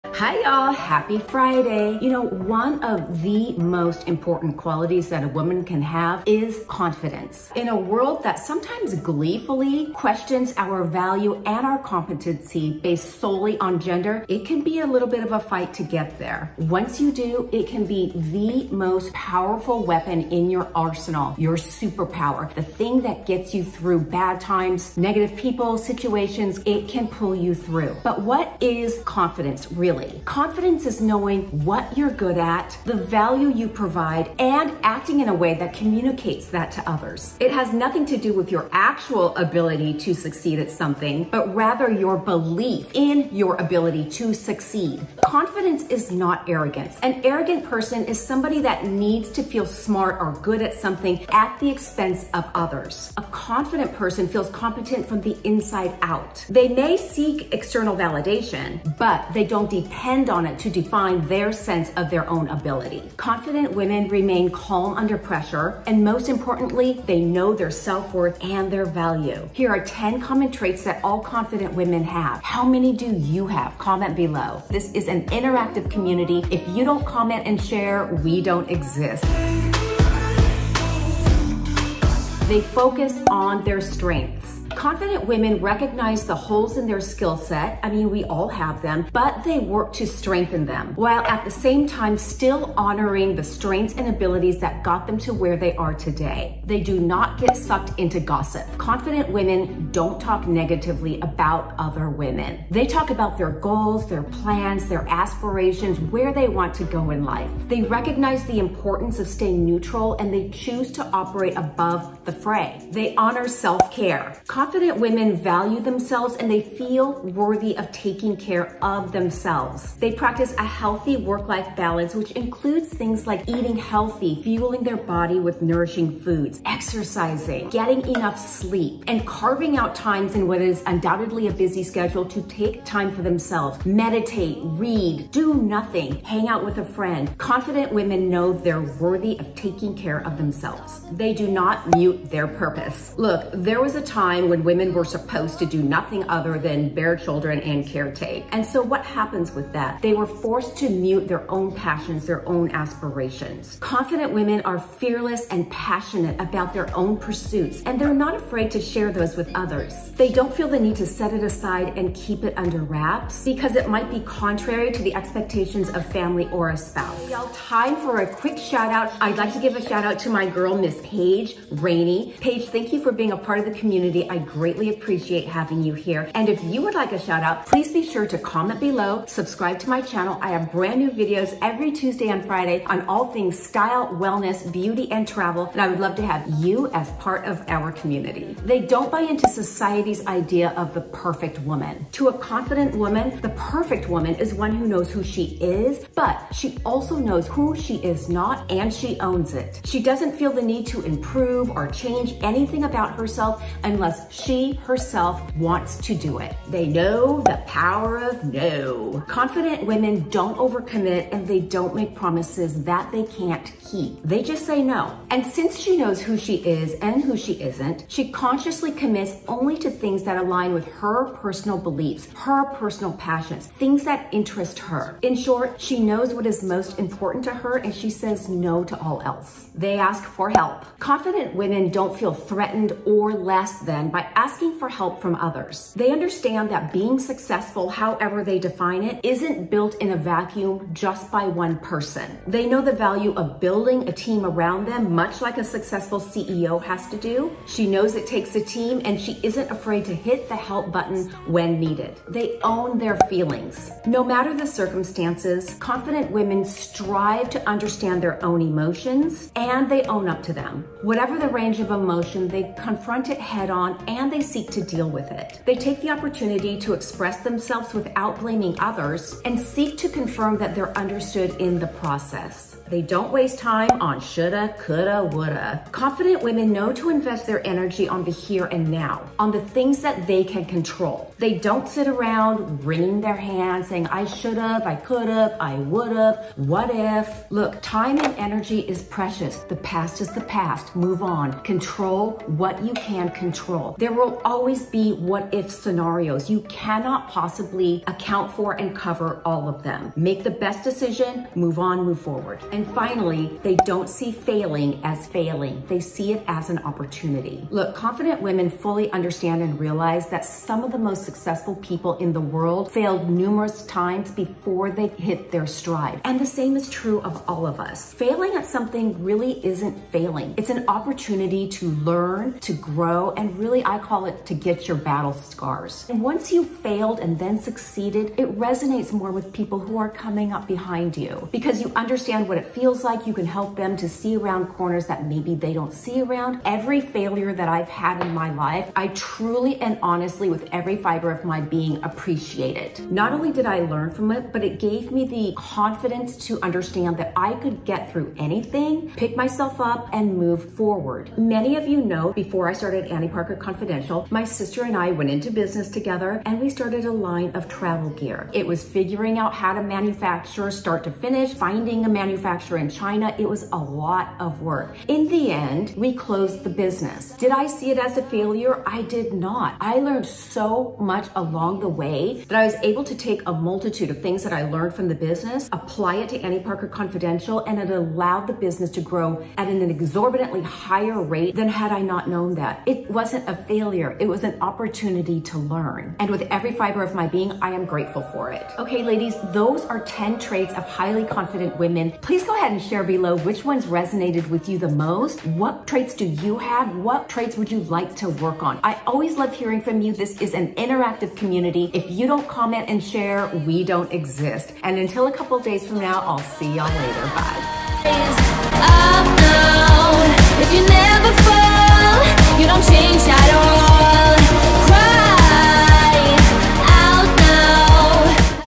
10 Traits of HIGHLY CONFIDENT WOMEN
Confident Women Share 10 Common Traits. How Many do YOU practice? Where would you like to BUILD more CONFIDENCE? annieparkerconfidential IG

My LIPCOLOR in Video:
+Tom Ford in 'blush nude'
+Chanel in '54 boy'
+LINER in 'pillow talk original'

My ORANGE TOP

VIDEO CHAPTERS:
0:00 Intro
0:27 What is CONFIDENCE
1:23 Focus On Strengths
1:36 They Don’t Gossip
1:52 Honor Self Care
2:21 Don’t Mute Their Purpose
2:47 SHOUT OUTS – is it you?
3:08 The ‘Perfect Woman’
3:26 The Power of NO
3:49 Ask for Help
4:12 Own Their Feelings
4:33 Don’t Waste Time
5:01 Failing as Opportunity
6:28 Outro